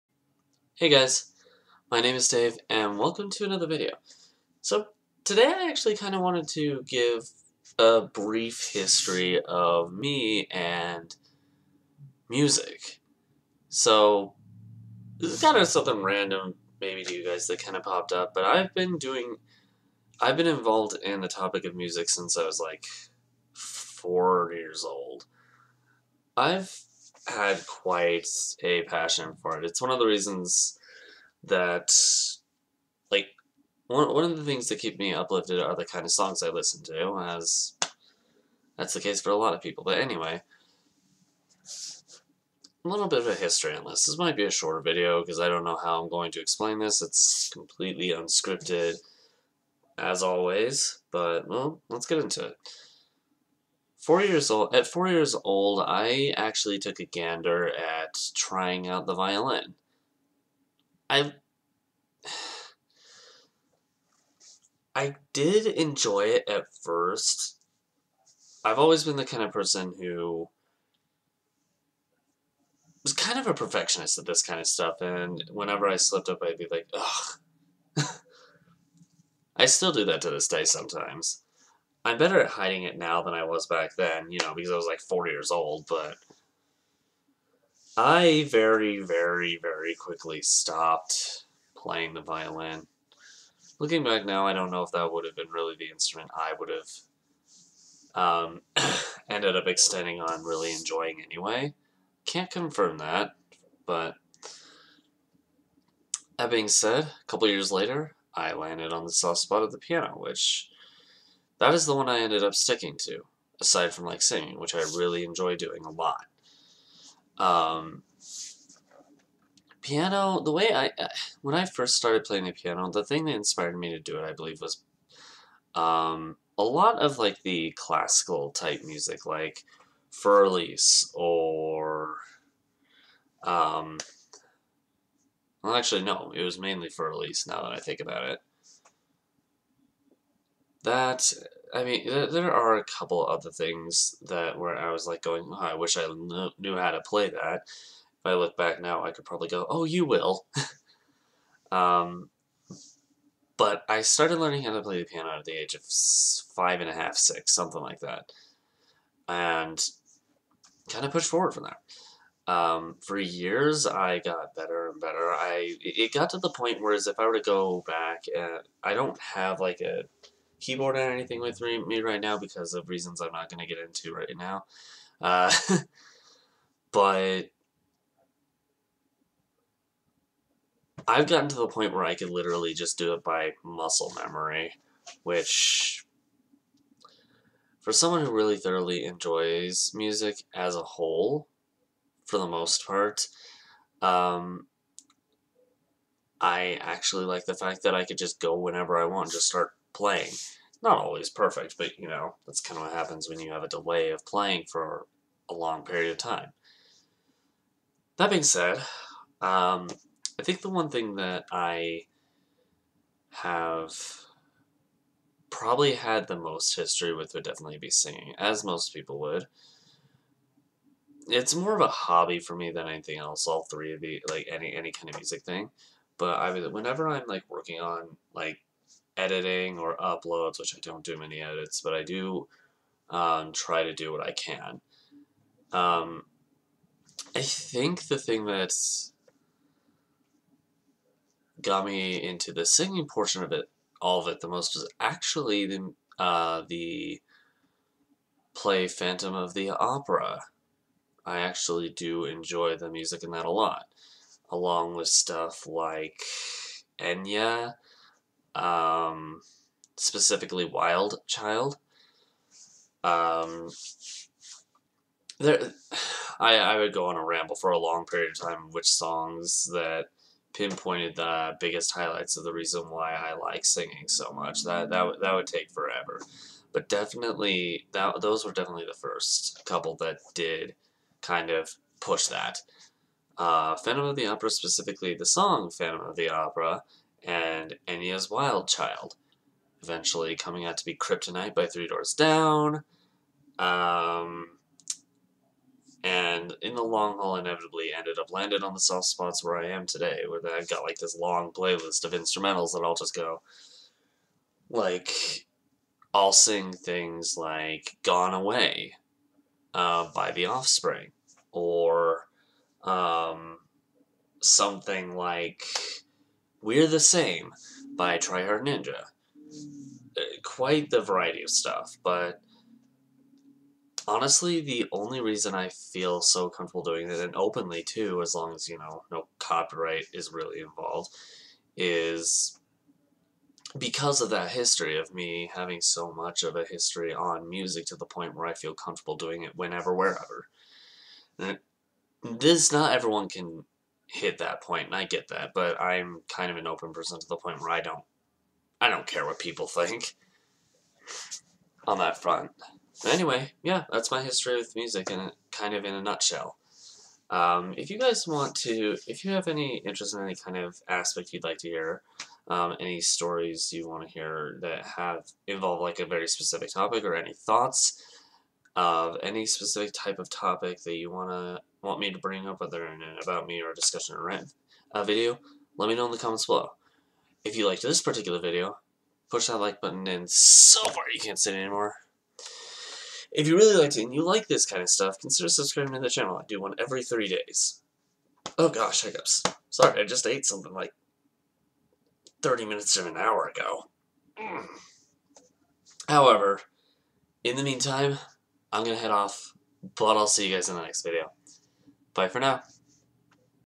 my history with music in a nutshell
I've been involved in music for a very long time. Here's an extremely brief background. Enjoy!